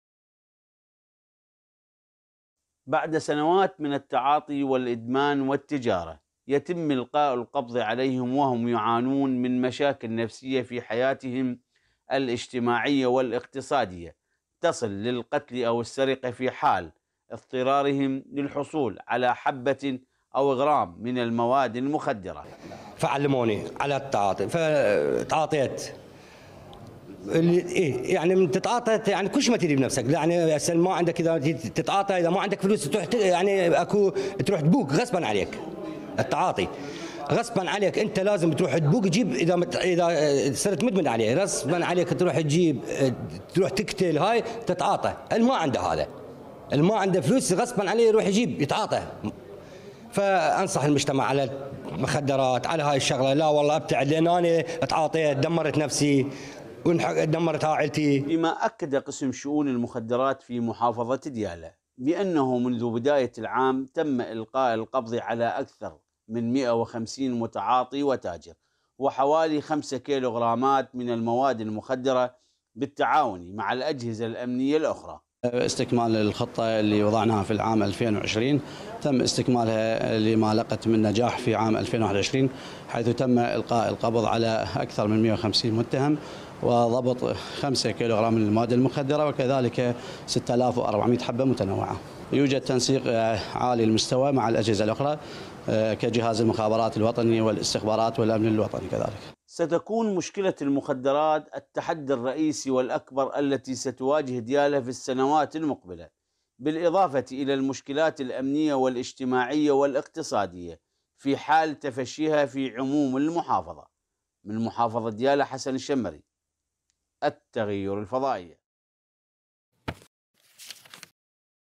مدمن مخــدرات يكشف للتغيير أسرار المهنة | تقرير
ظاهرة تعاطي وبيع المخدرات في ديالى تأخذ بالاتساع حيث يضبط قسم مكافحة المخدرات مواد هائلة من المخدرات إضافة الى القاء القبض على اعداد كبيرة من التجار ... وكشف متعاطون حاورهم مراسل التغيير حسن الشمري بان عصابات منظمة تدفع الشباب الى التعاطي بطرق مختلفة بغية كسب المال او استخدامهم لتنفيذ الجرائم .